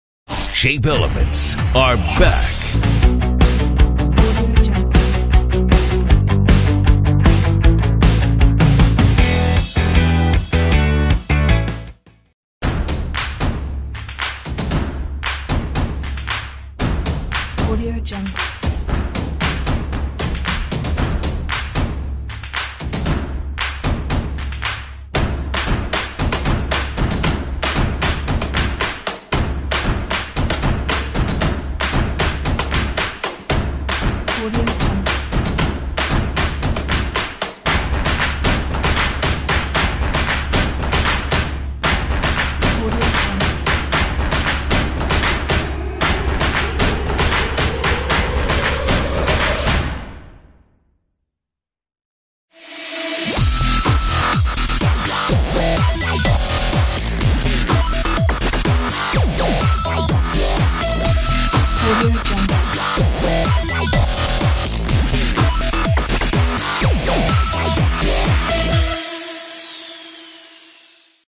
Typographic Opener Stomp | After Effects Template | Titles
Special Thank You ❤ to Jumadilovich!

Recommended Section:
0:01 Shape Elements 2
0:53 Download 'Chalk slideshow'
0:53 Download 'Handy Parallax Presets'
0:53 Music
1:01.200 Download 'Subscribe project'
Project does NOT require 3rd party plugins. There are 15 media  placeholders.  The price is $17.